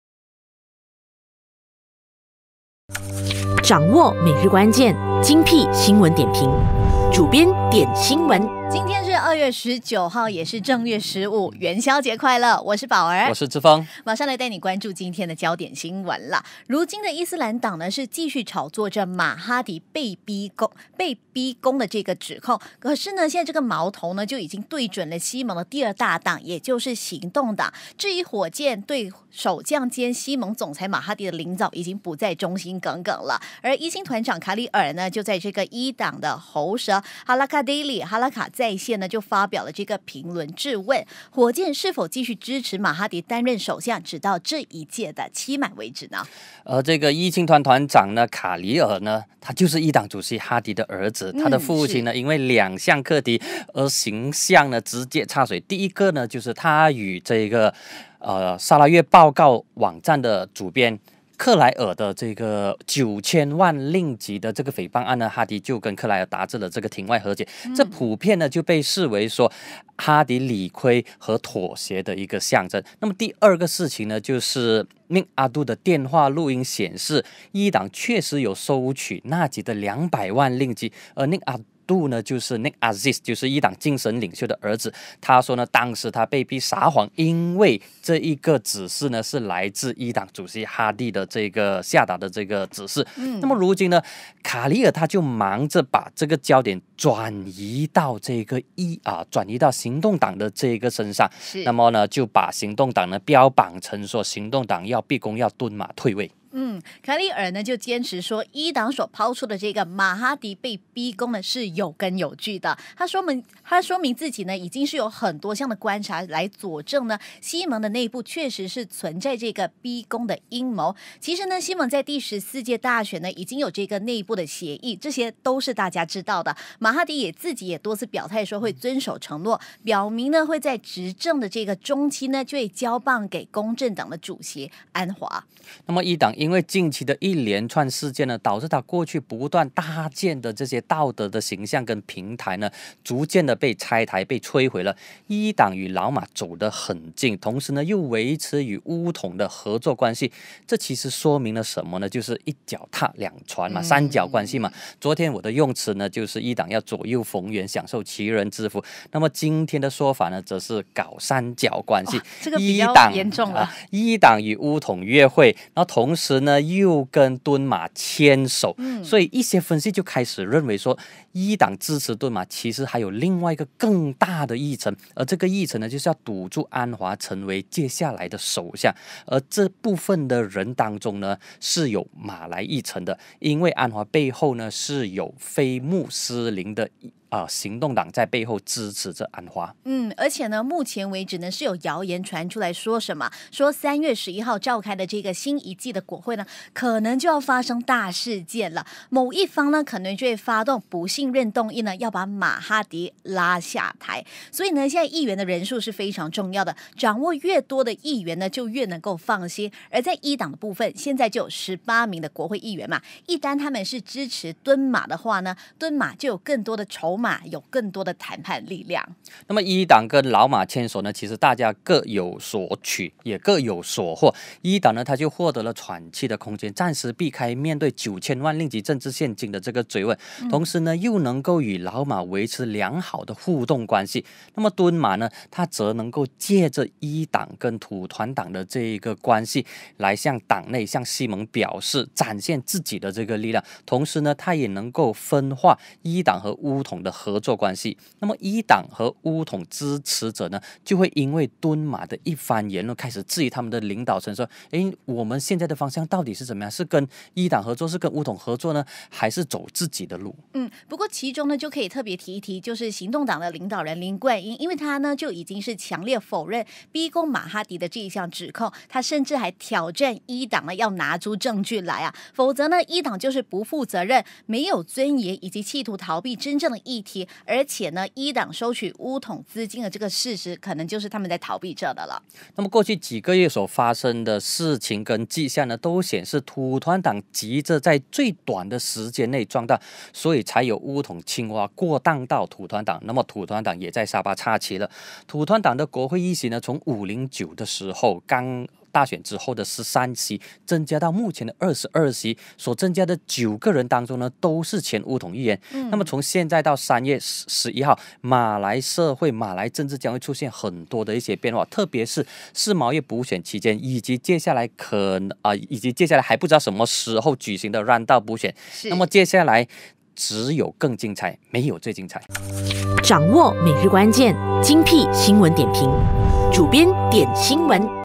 【主编点新闻 | 2019/2/19】伊党转移焦点 火箭成箭靶
伊斯兰党继续炒作“马哈迪被逼宫”指控，矛头对准行动党，质疑火箭对首相马哈迪领导的忠心。主编认为，过去几天马来政坛发生许多事件，从现在到3月11日的新一季国会开会前，马来政治预料会有更多变化，特别是士毛月补选期间，以及接下来的晏斗补选。